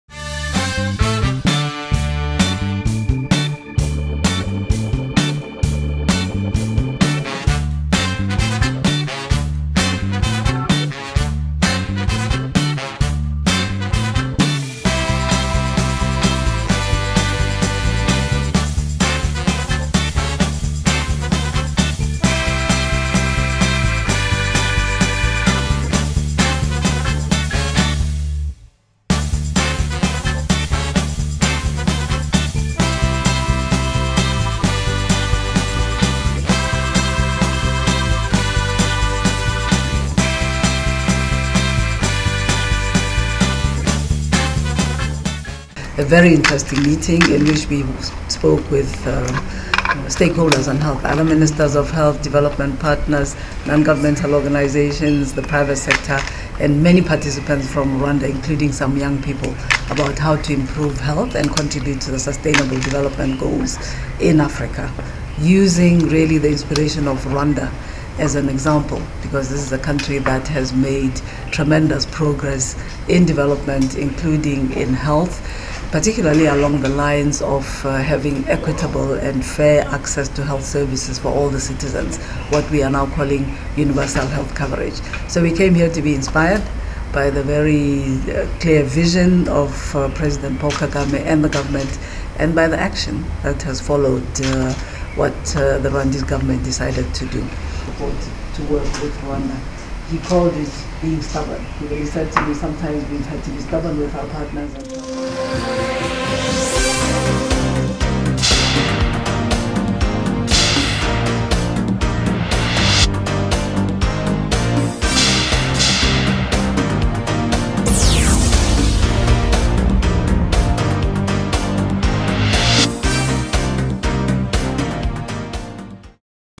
KAGAME MEETS Dr MATSHIDISO OF WHO
President Kagame on Thursday met and held talks with Dr Matshidiso Rebecca Moeti, the Regional Director for WHO's Africa Region at Urugwiro Village.

Camera: Urugwiro Village